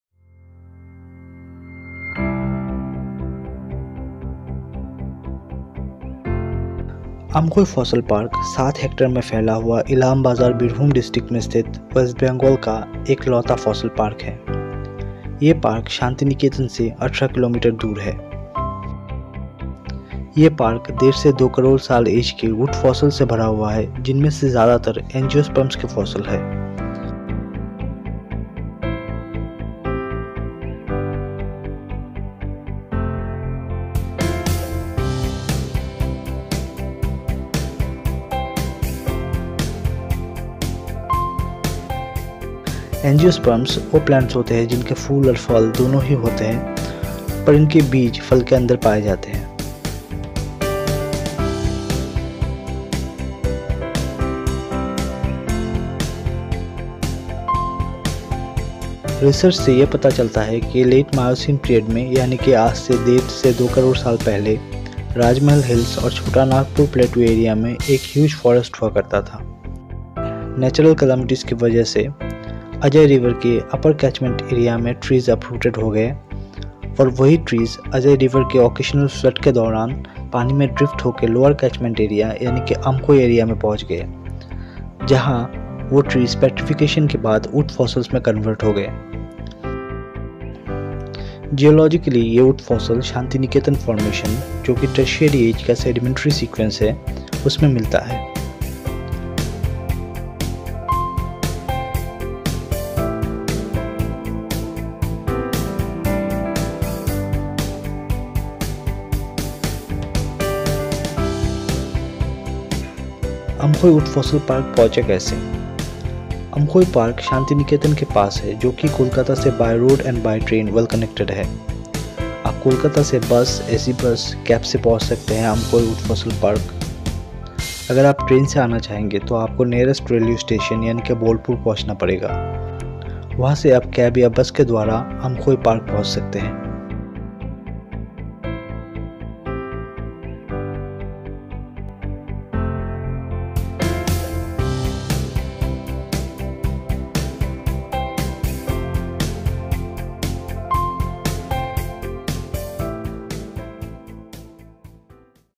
Amkhoi wood fossil park | Best adventurous places in India |Geotourism places| Budget travel
Amkhoi wood fossil park | Best adventurous places in India |Geotourism places| Adventure travel|

Amkhoi wood fossil park is situated near Illambazar, Birbhum district of West Bengal. This park is 18 kms away from Shantiniketan (Bolpur).
During excavation of ponds fossil woods were discovered from this area. These wood fossils are 10 to 20 million years old, believed to be late Miocene age.
Geologically the fossil are found in the Shantiniketan formation, which is a sedimentary sequence of Early Tertiary age.
Amkhoi wood fossil park is well connected to Kolkata by road and train, one can find Bus or hire cab from Kolkata to reach Amkhoi. Everyday numerous trains from both Howrah and Sealdah stations runs upto Bolpur. If you wish to visit Amkhoi wood fossil park by train then you have to reach the nearest railway station Bolpur Junction, from Bolpur one can hire taxi or take a bus ride to reach Amkhoi which is a 18 kms ride from shantiniketan.